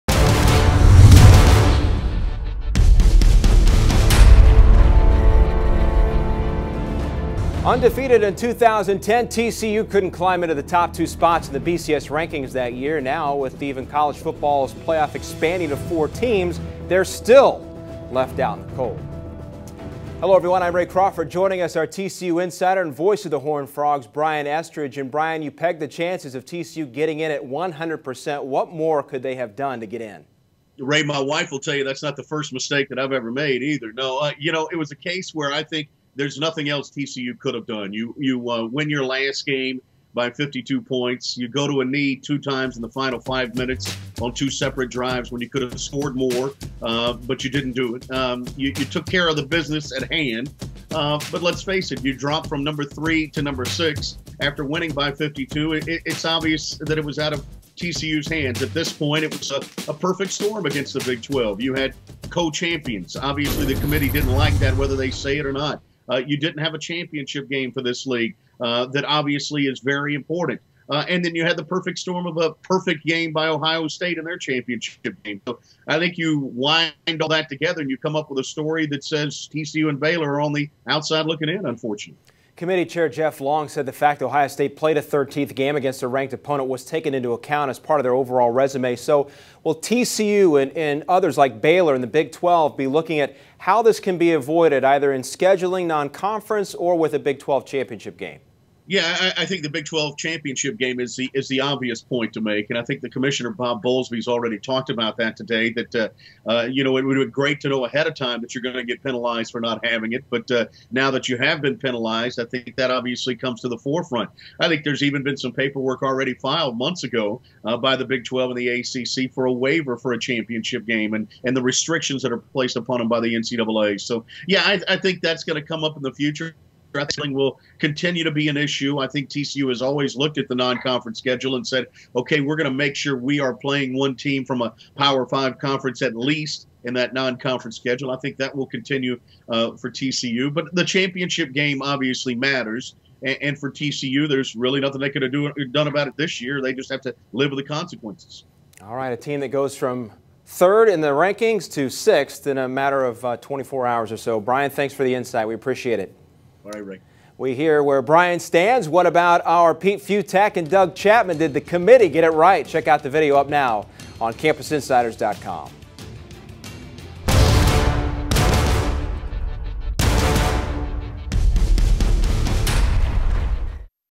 College Football Playoff: TCU Insider Reaction | CampusInsiders
Despite an 11-1 record, the Horned Frogs did not make the cut for the College Football Playoff.  Our Insider Brian Estridge offers perspective on what this means for the program.